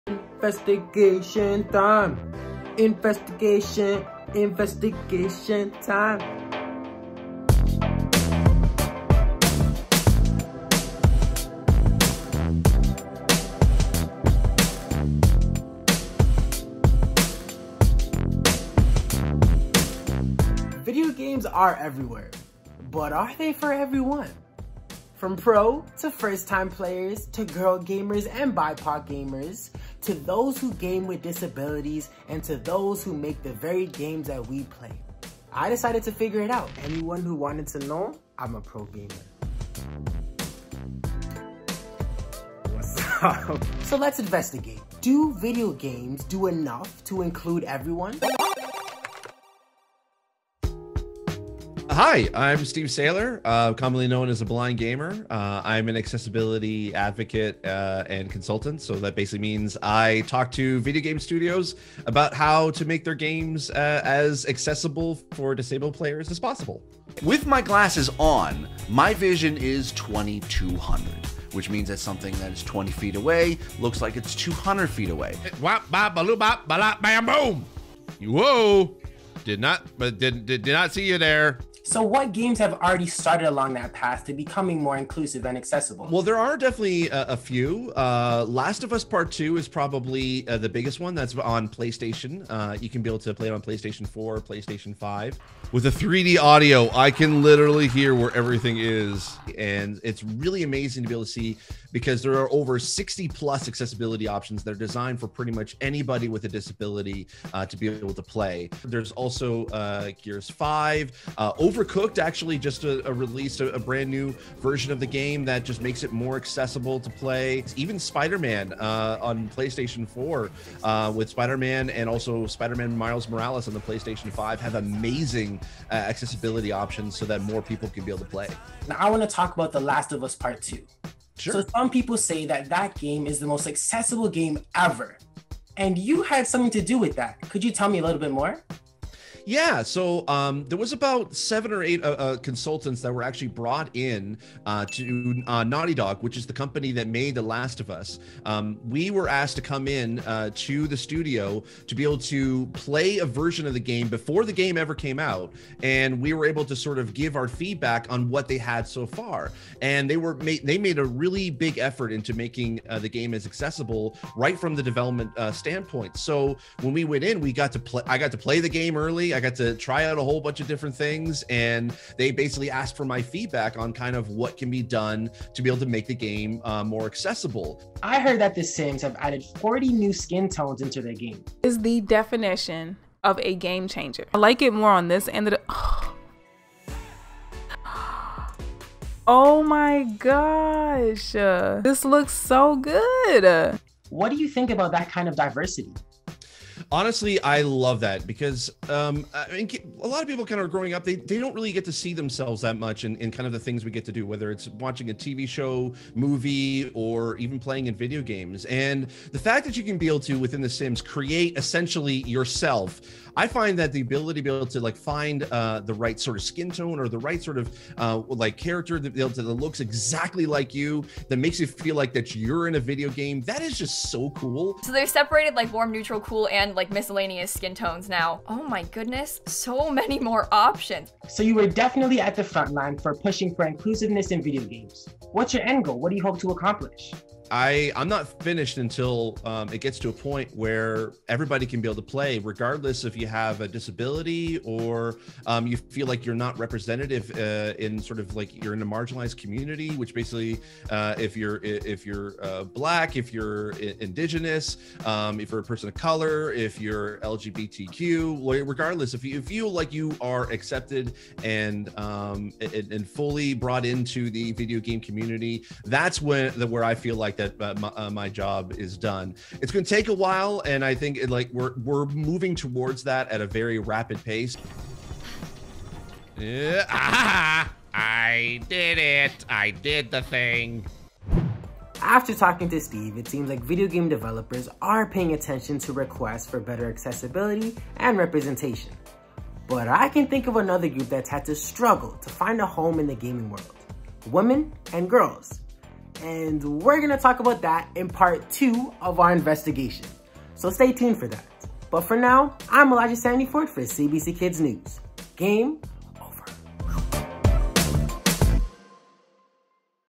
Do video games do enough to include everyone? | CBC Kids News
It feels like video games are everywhere — on our phones, laptops or PCs, gaming consoles — we can’t get enough.

And even though it may seem like gaming options are endless, for some people, that's not the case.

Some gamers with disabilities feel left out from the gaming experience and gamers from diverse backgrounds often don’t see themselves reflected on screen.

So, are video games for everyone?

In part one, Elijah talks to Steve Saylor, a blind video gamer and game consultant all about inclusion and diversity in gaming.

CBC Kids News is a website for kids, covering the information you want to know. Real Kids. Real News. Check it out at CBCKidsNews.ca.